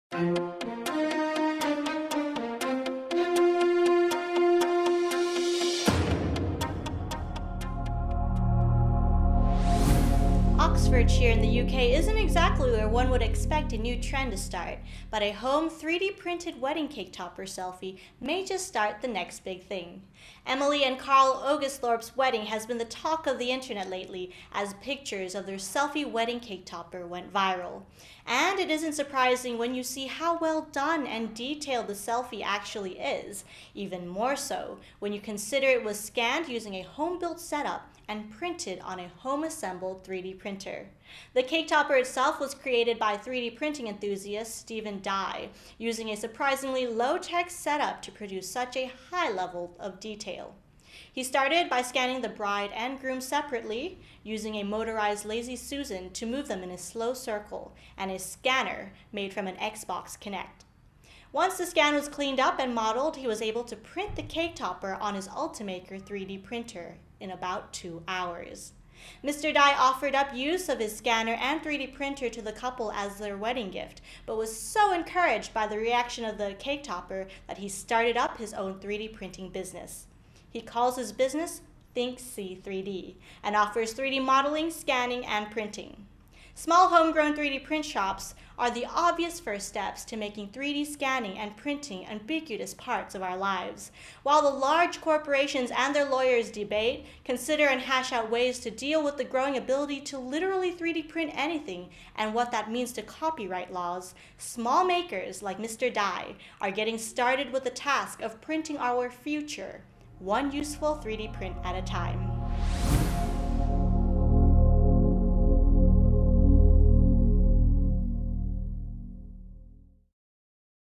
Small 3D Printing Businesses Cut Themselves a Slice of the Cake
Transcript: Oxfordshire in the UK isn't exactly where one would expect a new trend to start, but a home 3D printed wedding cake topper selfie may just start the next big thing.
Emily and Carl Ogasthorp's wedding has been the talk of the internet lately, as pictures of their selfie wedding cake topper went viral.
And it isn't surprising when you see how well done and detailed the selfie actually is.
Even more so when you consider it was scanned using a home built setup and printed on a home-assembled 3D printer.
The cake topper itself was created by 3D printing enthusiast Steven Dey using a surprisingly low-tech setup to produce such a high level of detail.
He started by scanning the bride and groom separately, using a motorized lazy susan to move them in a slow circle and a scanner made from an X-Box Kinect.
Once the scan was cleaned up and modelled he was able to print the cake topper on his Ultimaker 3D printer in about two hours.
Mr Dey offered up use of his scanner and 3D printer to the couple as their wedding gift, but was so encouraged by the reaction to the cake topper that he's started up his own 3D printing business.
He calls his business ThinkSee3D and offers 3D modelling, scanning and printing.
Small homegrown 3D print shops are the obvious first step to making 3D scanning and printing ubiquitous parts of our lives.
While the large corporations and their lawyers debate, consider and hash out ways to deal with the growing ability to literally 3D print anything and what that means for copyright laws, small makers like Mr. Dey are getting started with the task of printing our future one useful 3D print at a time.